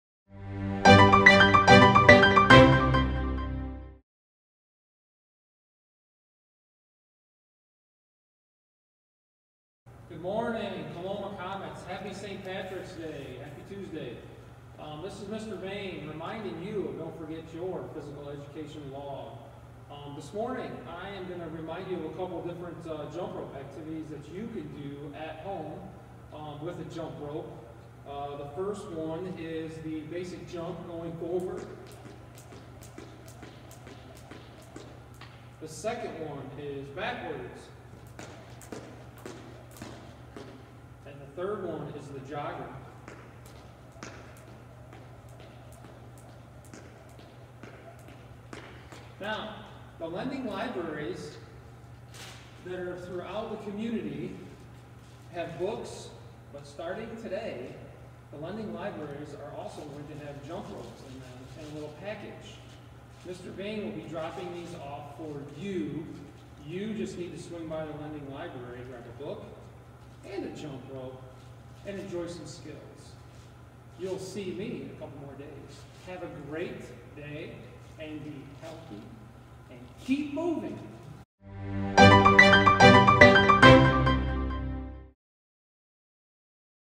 Keep Moving with Mr. Veine - Episode 1
Check out these physical education tips from Mr. Veine to help you KEEP MOVING!